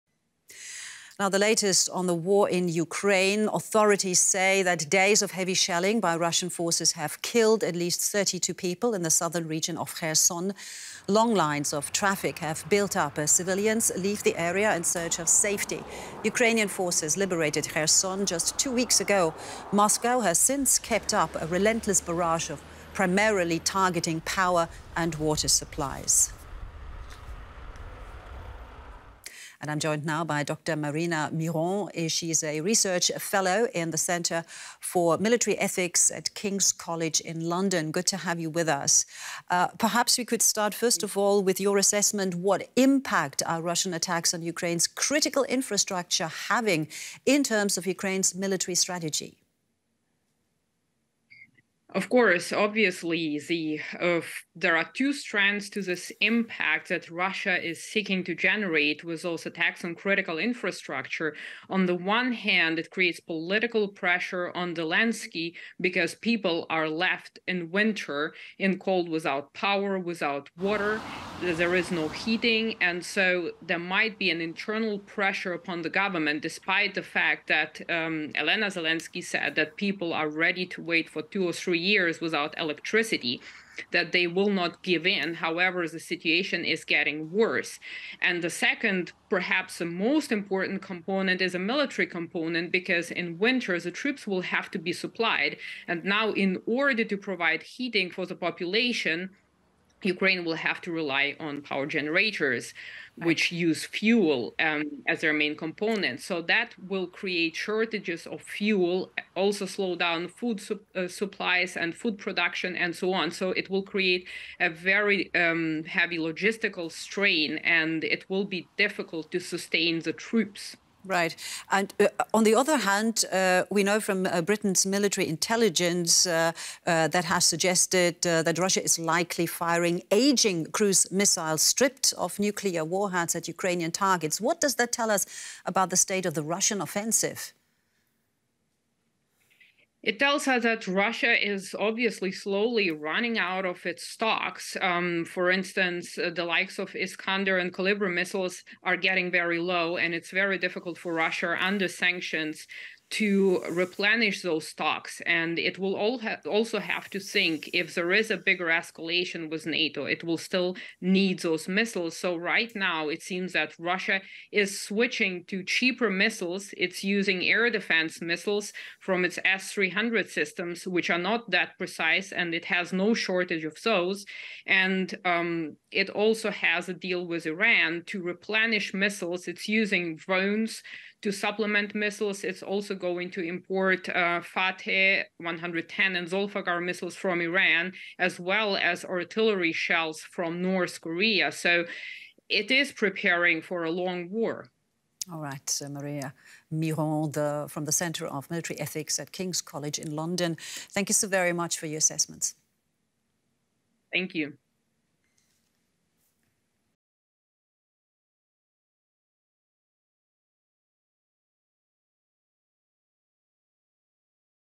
What is the state of Russia's offensive in Ukraine? | DW News
Authorities say days of heavy shelling by Russian forces have killed  at least 32 people in the southern region of the Ukrainian city of Kherson.
Long lines of traffic have built up as civilians leave the area in search of safety. Ukrainian forces liberated Kherson just two weeks ago. Moscow has since kept up a relentless barrage primarily targeting power and water supplies.
But reports suggest that Russia is increasingly relying on imprecise, ageing cruise missiles, meaning that it has run out of modern weapons. DW spoke with military analyst Marina Miron of King's College London for an assessment of the situation.